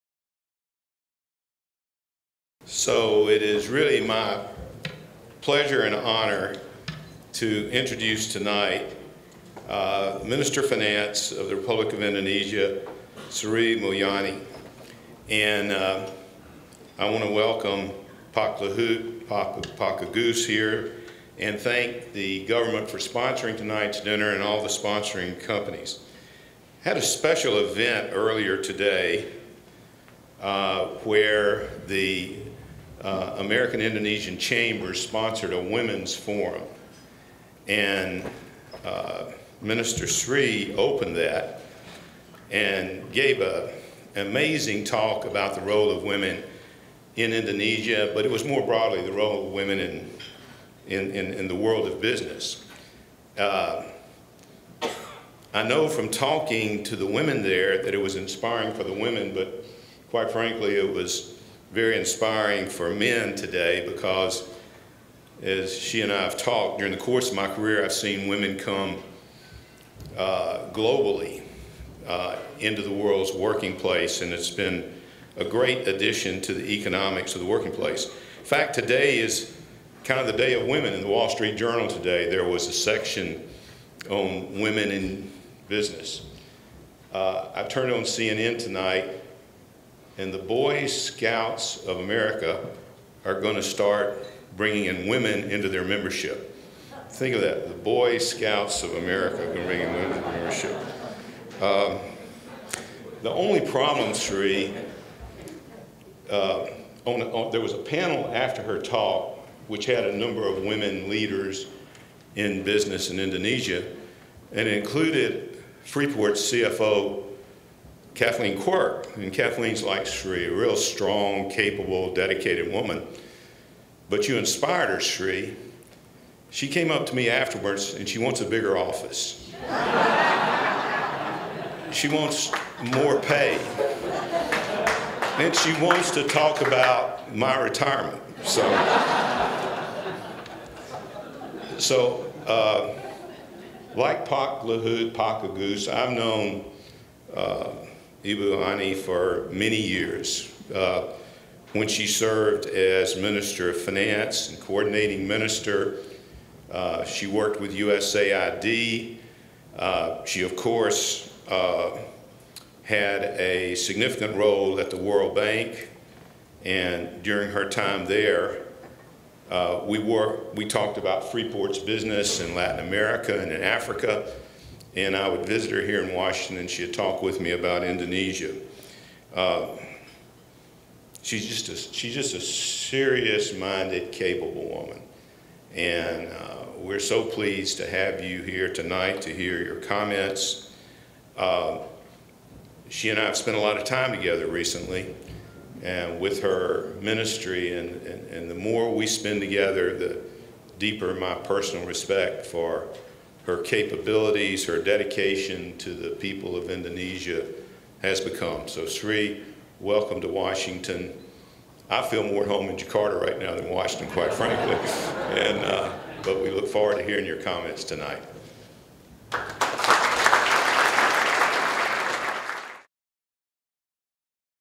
Voyage to Indonesia Gala Dinner- Richard Adkerson
Introduction of Finance Minister Sri Mulyani by Richard Adkerson